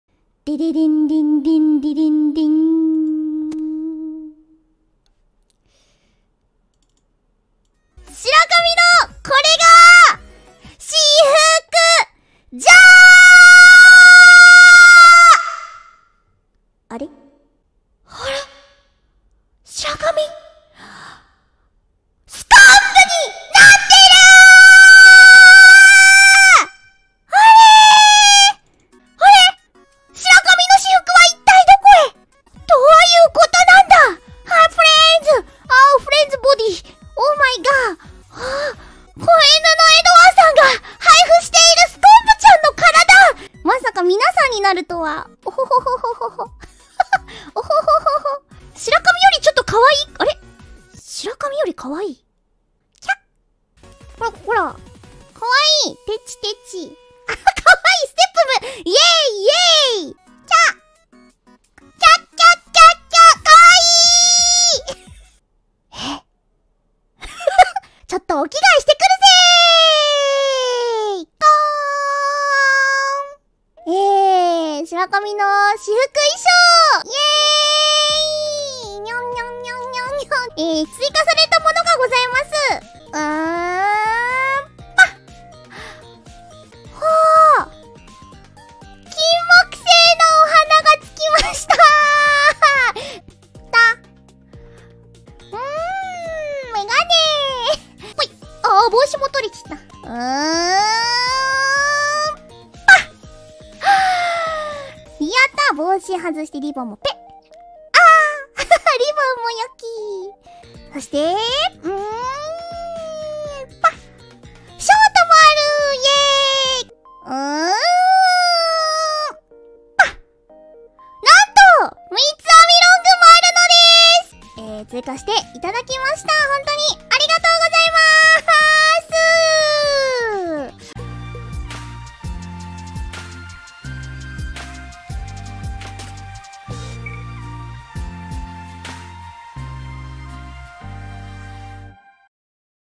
Fubuki Reveal Sukonbu's Live 2D Model!!! New Accessories & Hair【Hololive | Eng Sub】
Fubuki reveals Sukonbu's avatar made and shared by fans along with her new accessories and hair added to her casual outfits.

※No re-uploading of my videos on any platforms.